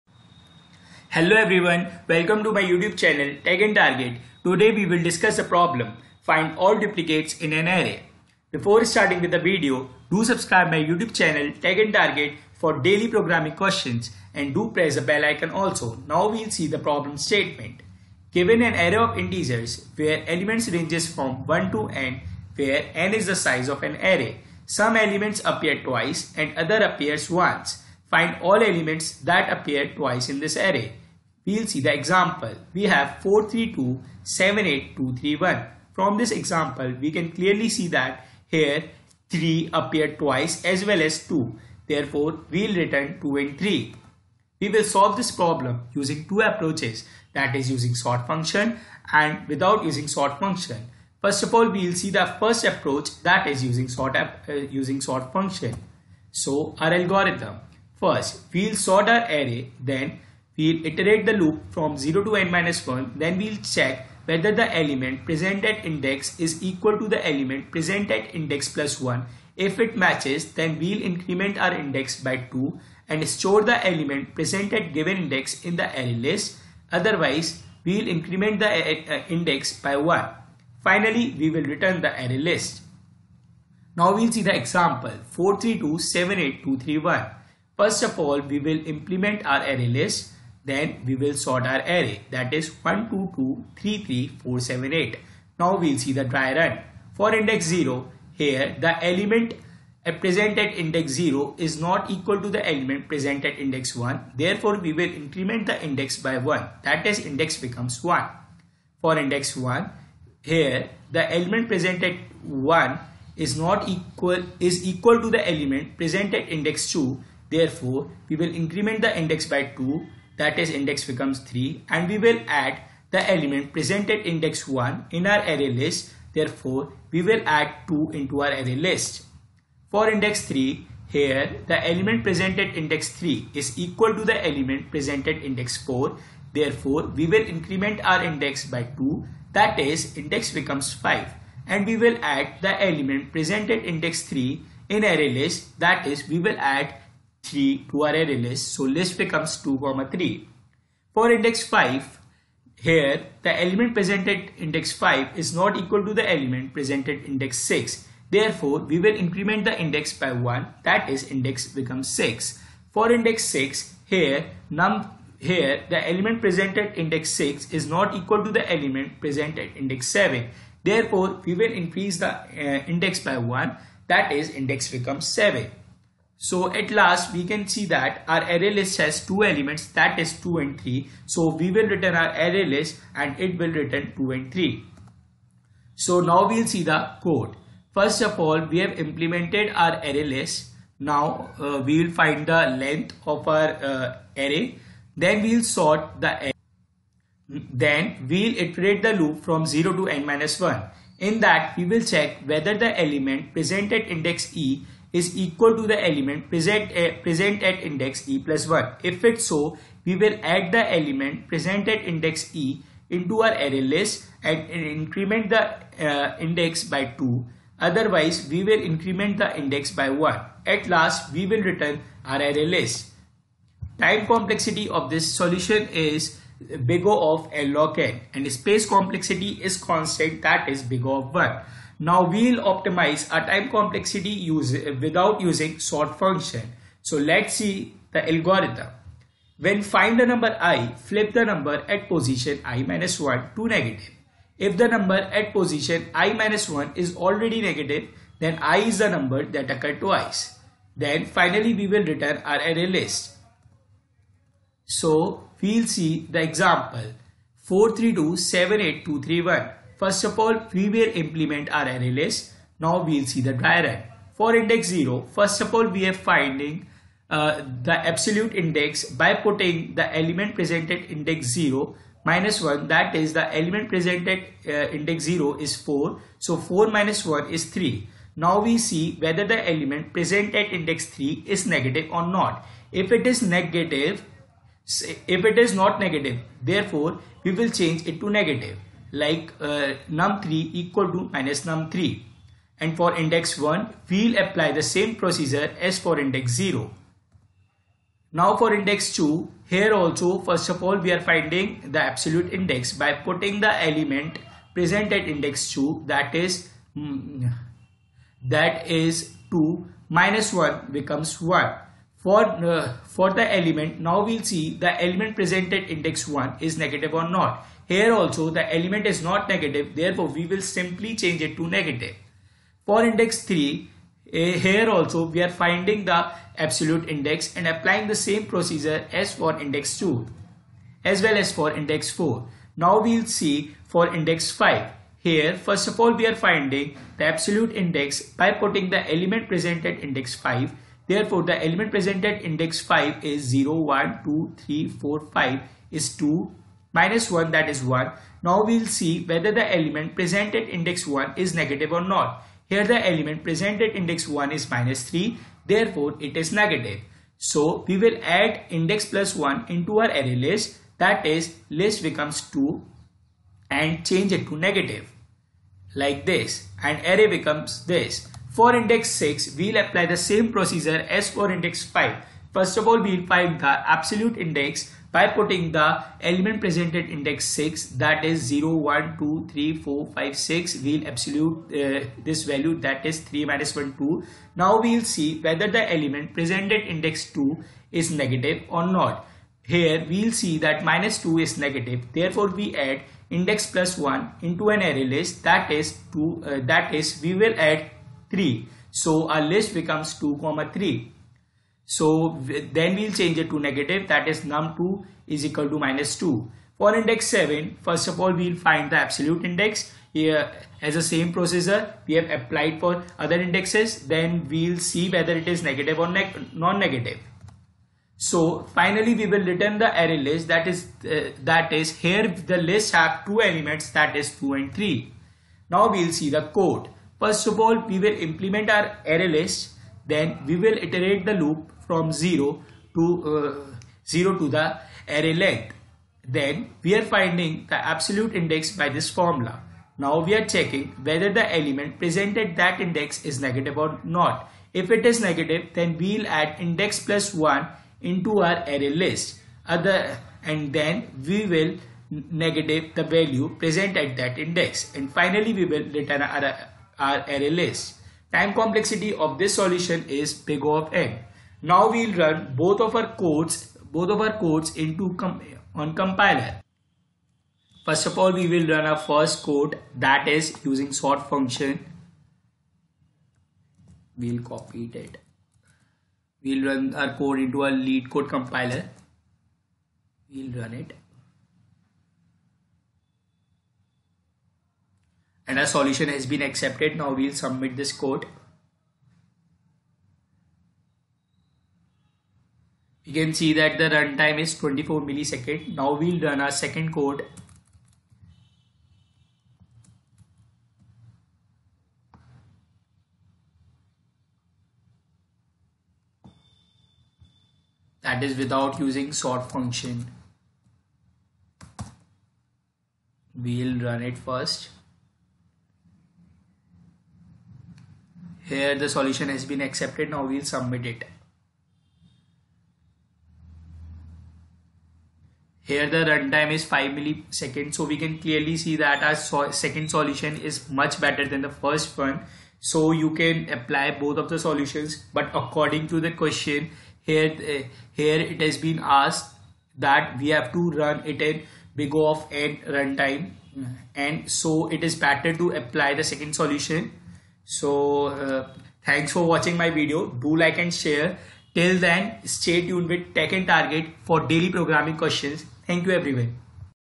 Find all Duplicates in an Array | Leetcode 442 | Two Approaches | Simple and Easy Logic
Hello Everyone

Lets start August Challenge Day 6!!

In this video i have discussed the leetcode problem 442 i.e Find all Duplicates in an Array